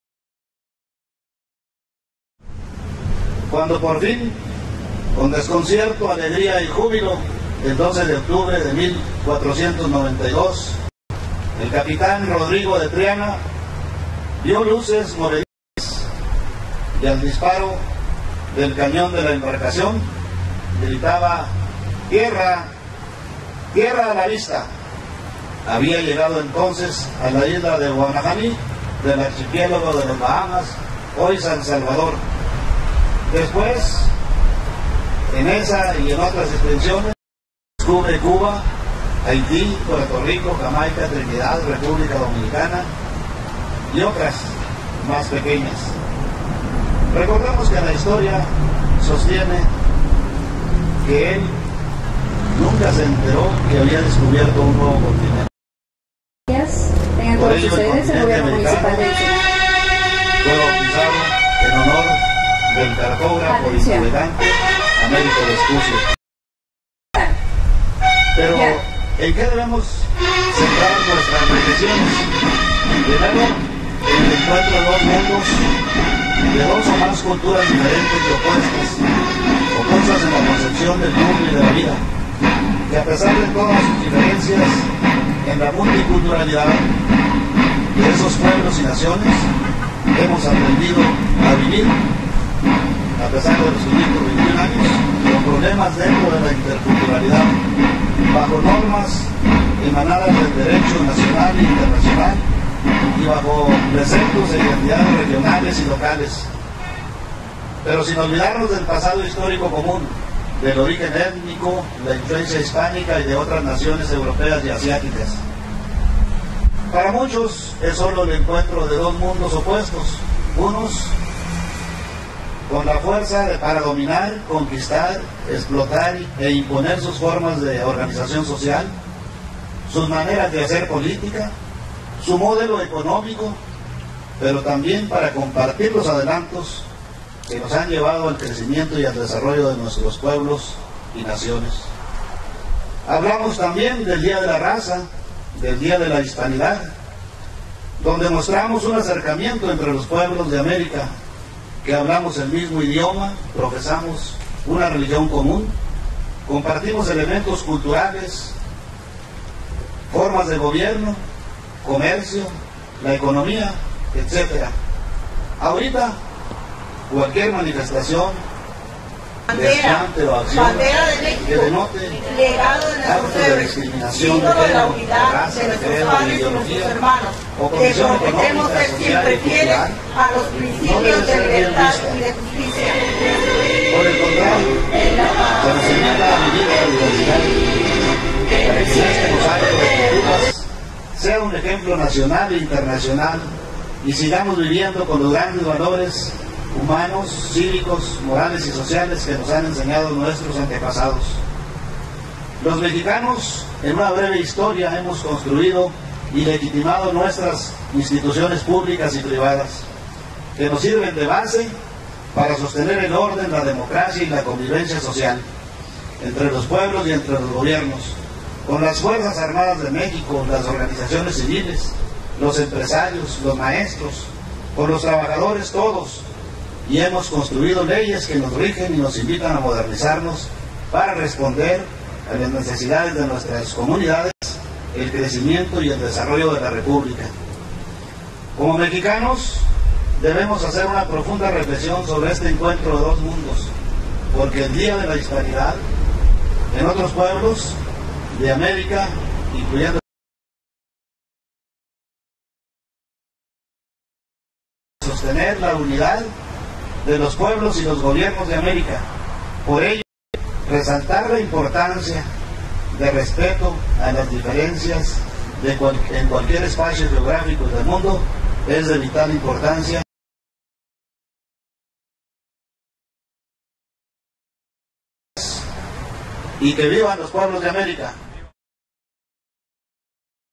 521 años de arribo de Cristóbal Colón a América
En un acto al cual sólo asistieron funcionarios y empleados municipales, y que tuvo lugar en el Riviera del Pacífico, se conmemoró el 521 aniversario del arribo de Cristóbal Colón a tierras del continente americano.

El orador oficial, René Bustillos Díaz, coordinador del Servicios de Apoyo de la Dirección Municipal de Educación y Cultura, dijo en su discurso que para muchos es sólo el encuentro de dos mundos opuestos; uno con la fuerza para dominar, conquistar, explotar e imponer sus formas de organización social, sus maneras de hacer política, su modelo económico, así como para compartir los adelantos que nos lleven al crecimiento y al desarrollo de nuestros pueblos y naciones.